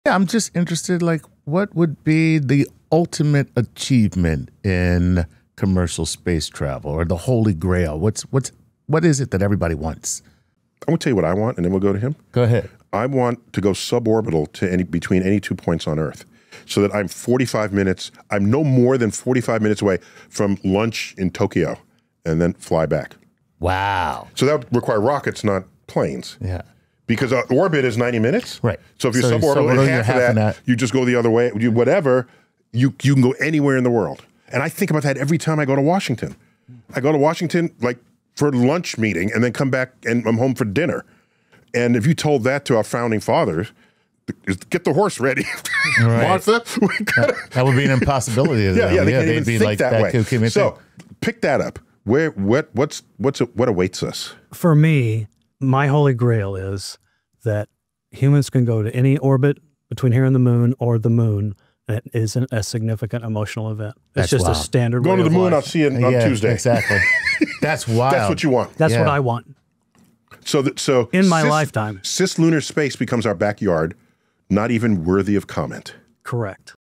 Commercial Space Travel: The Ultimate Achievement?
What's the ultimate goal in commercial space travel? Imagine a future where lunar visits are routine and any spot on Earth is just a 45-minute rocket ride away. That's the dream.